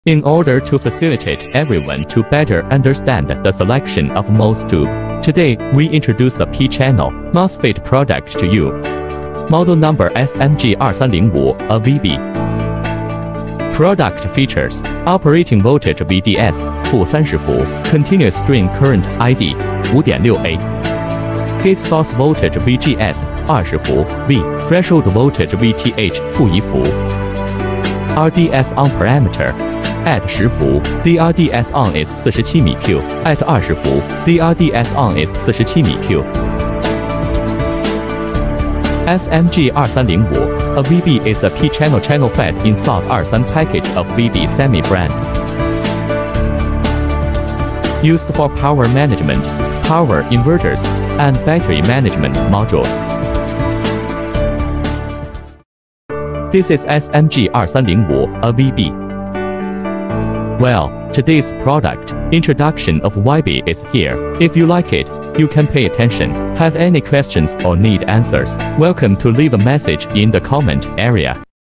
SMG2305A-VB is a SOT23 package P-Channel field effect MOS tube
"SMG2305A-VB is a P-Channel field effect transistor in SOT23 package of VBsemi brand. The following are detailed parameters and application introduction:

- Parameters:
- Operating voltage (VDS): -30V
- Continuous drain current (ID): -5.6A
- On-state drain-source resistance (RDS(ON)): 47mΩ@VGS=10V, VGS=20V
- Threshold voltage (Vth): -1V

- Package: SOT23

This transistor is suitable for the following fields and modules:

1. **Power management module:** Due to its P-Channel channel characteristics and moderate current capacity, it can be used in power switches and power management modules.

2. **Power Inverter:** Suitable for power inverter, especially in applications where P-Channel MOSFET is required.

3. **Battery Management System:** Due to low leakage current and moderate current capacity, it can be used in battery management system to control battery charging and discharging.

Please note that the specific application and module selection may vary according to the specific design requirements and environment. It is recommended to refer to the data sheet and specification book in the specific application."